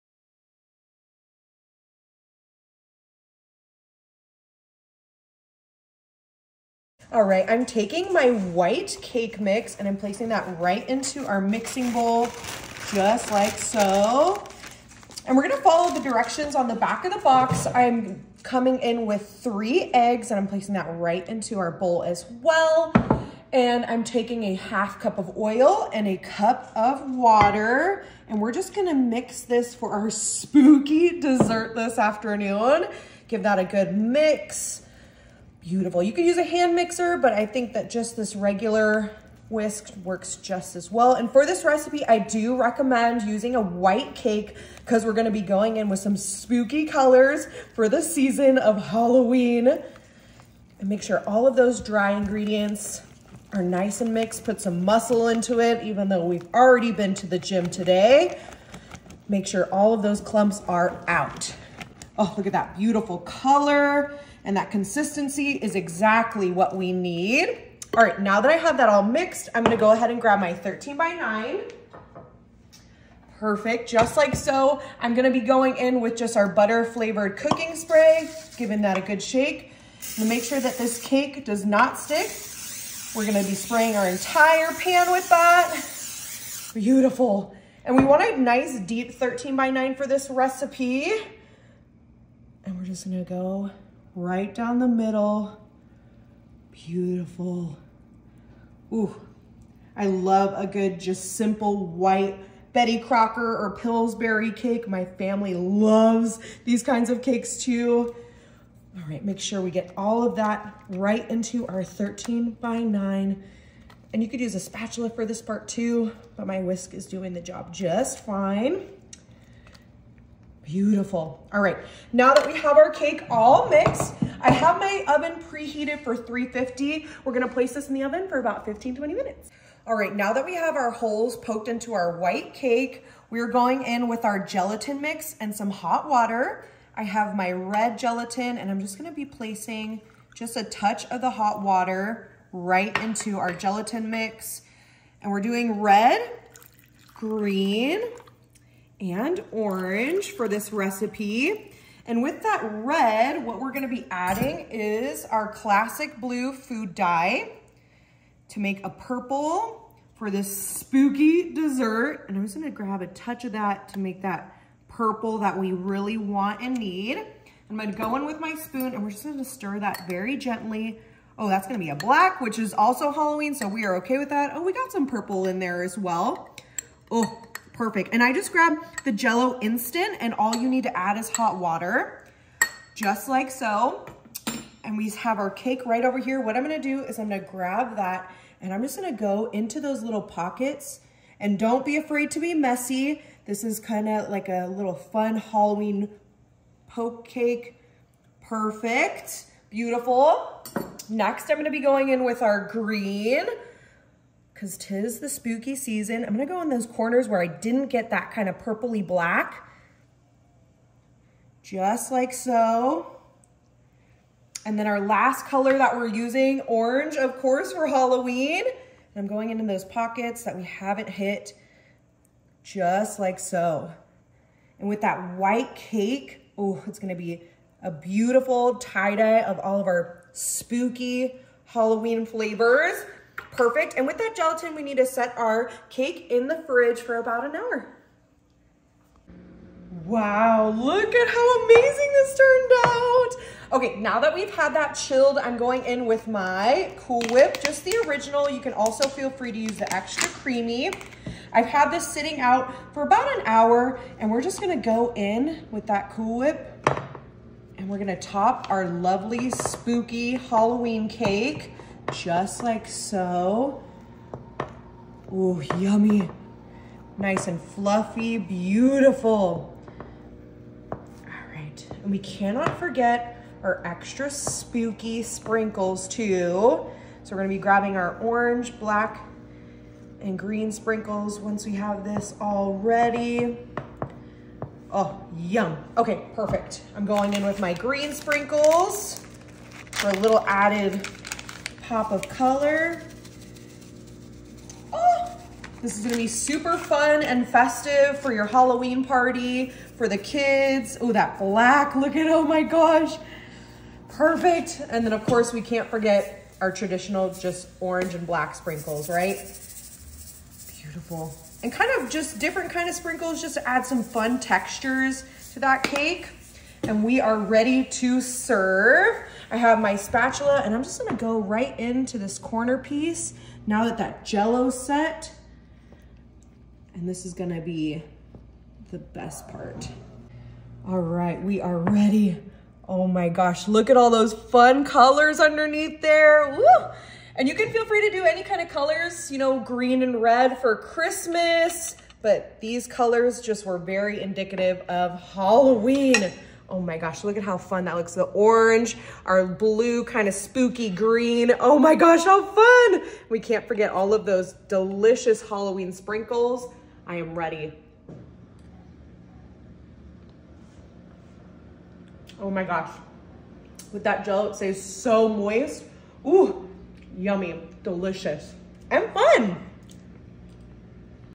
Easy spooky treat!
I make a Halloween poke cake that is perfect for the kids